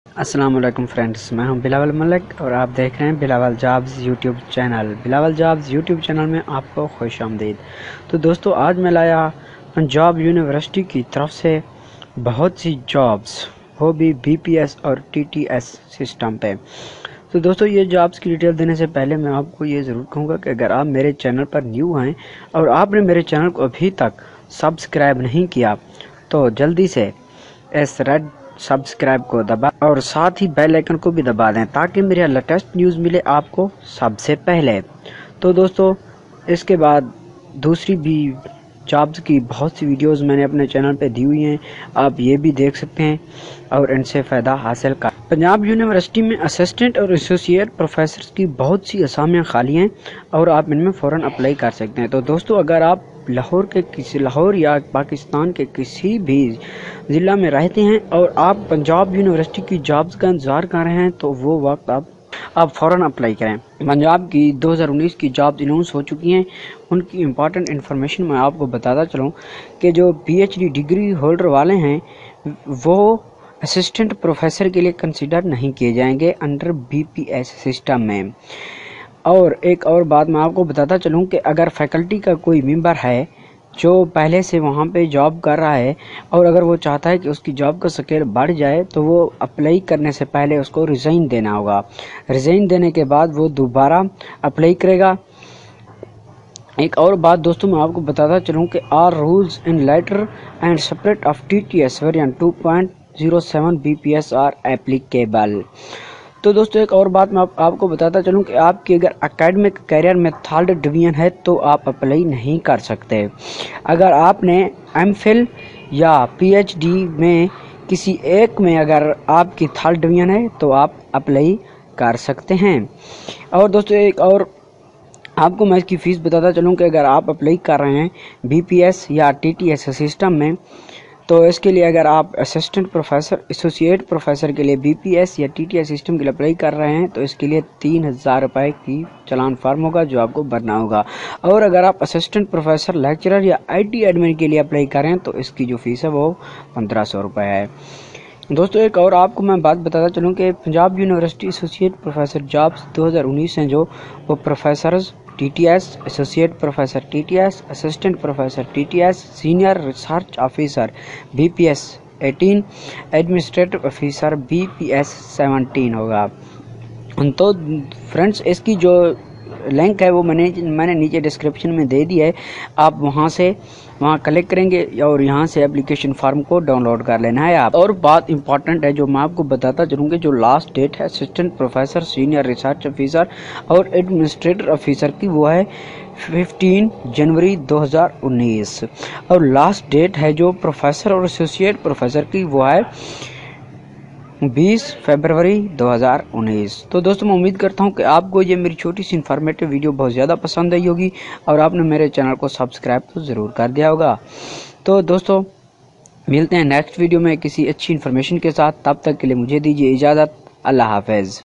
university of punjab associate,assistant professor jobs 2019||Bilawal Jobs
Hey friends:
here are university of punjab associate,assistant professor jobs  2019

Ph.D. degree holder will not be considered for Assistant Professor under BS system, whereas in-service Punjab University Ph.D's may apply on BS.
Foreign degree holders must be provided equivalence certificate issued by the HEC
All rules in letter & spirit of TTS version 2.0 & BS are applicable.
The candidates applying for the posts of Professor/Associate Professor Under BS/TTS should submit six copies of applications alongwith six complete sets of testimonials/ research work/publications/ photograph (1x1)/ certificate of any distinction etc., on the relevant prescribed form .
The candidates applying for the posts of Assistant Professor/Lecturer Under BS/TTS are required to submit one copy of application on the relevant prescribed form alongwith attested copies of degrees/certificates/testimonials/ photograph (1x1) / certificate of any distinction etc.
Candidates applying for the posts of Professor/Associate Professor under BS/TTS are required to submit a crossed postal order/ bank draft of Rs.3000/ -. The candidates applying for the posts of Assistant Professor/Lecturer u nder BS/TTS are however, required to submit a crossed postal order/ bank draft of Rs.1500/-. The postal order/bank draft should be drawn in favour of the Treasurer, University of the Punjab, Lahore.